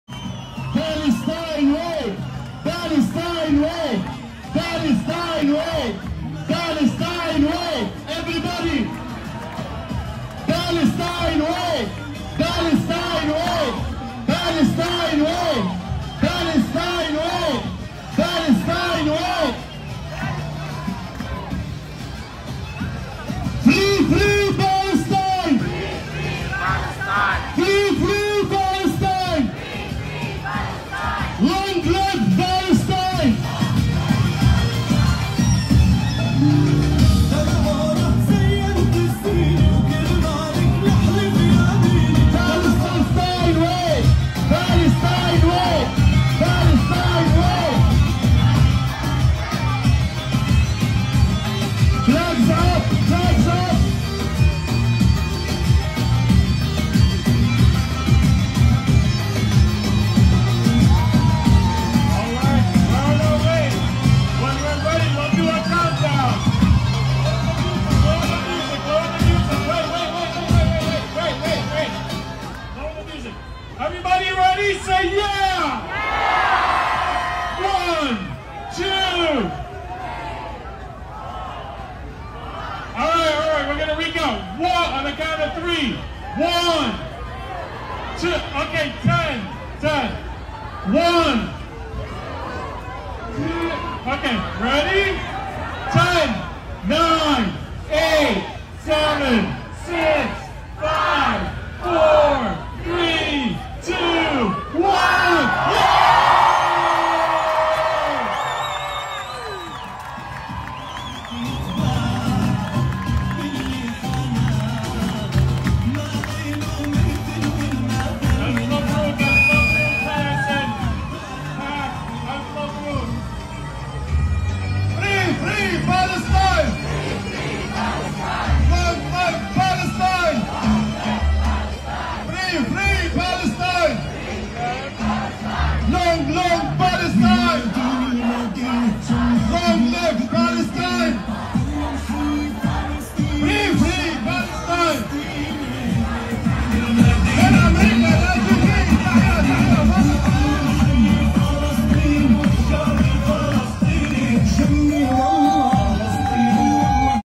تدشين طريق بإسم فلسطين 🇵🇸في امريكا 🇺🇸palestine way
لأول مرة يدشن طريق بإسم فلسطين 🇵🇸 في الولايات المتحدة الاميركية🇺🇸